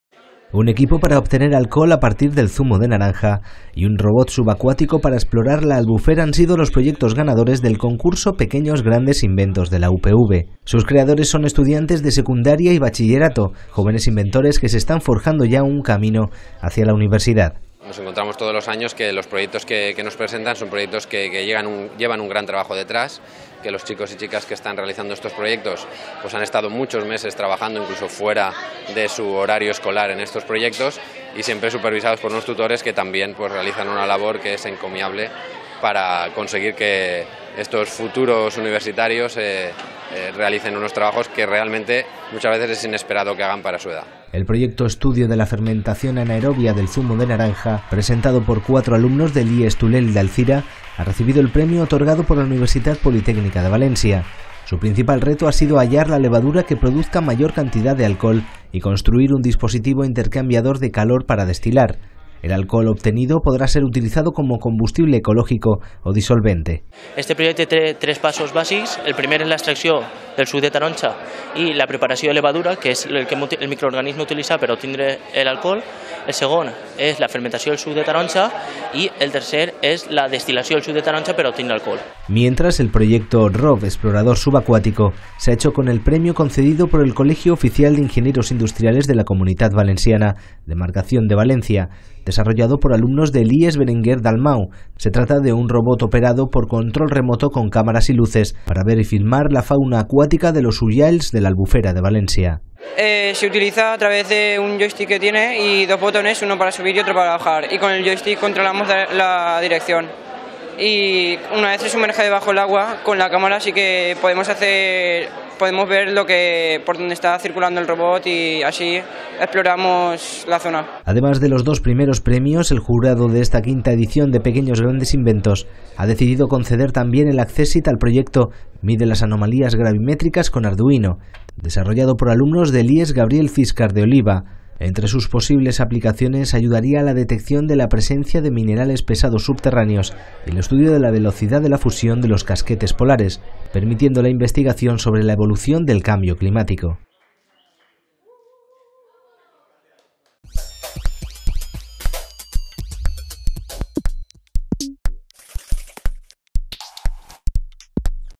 Noticia @UPVTV: Pequeños Grandes Inventos 2015 [04-06-2015]
Un equipo para obtener alcohol a partir del zumo de naranja y un robot subacuático para explorar la Albufera han sido los proyectos ganadores del concurso Pequeños Grandes Inventos de la UPV. Sus creadores son estudiantes de secundaria y bachillerato. Jóvenes inventores que se están forjando ya un camino hacia la universidad.

El proyecto “Estudio de la fermentación anaerobia del zumo de naranja”, presentado por cuatro alumnos del IES Tulell de Alzira ha recibido el premio otorgado por la Universitat Politècnica de València. Su principal reto ha sido hallar la levadura que produzca mayor cantidad de alcohol y construir un dispositivo intercambiador de calor para destilar. El alcohol obtenido podrá ser utilizado como combustible ecológico o disolvente.

Mientras, el proyecto “ROV explorador subacuático” se ha hecho con el premio concedido por el Colegio Oficial de Ingenieros Industriales de la Comunitat Valenciana - Demarcación de Valencia. Desarrollado por alumnos del IES Berenguer Dalmau de Catarroja. Se trata de un robot operado por control remoto con cámaras y luces, para ver y filmar la fauna acuática de los “ullals” de la Albufera de Valencia

Además de los dos primeros premios, el jurado de esta quinta edición de Pequeños Grandes Inventos ha decidido conceder también un accésit al proyecto “Mide las anomalías gravimétricas con Arduino”. Desarrollado por alumnos del IES Gabriel Ciscar de Oliva, entre sus posibles aplicaciones ayudaría a la detección de la presencia de minerales pesados subterráneos y el estudio de la velocidad de la fusión de los casquetes polares, permitiendo la investigación sobre la evolución del cambio climático.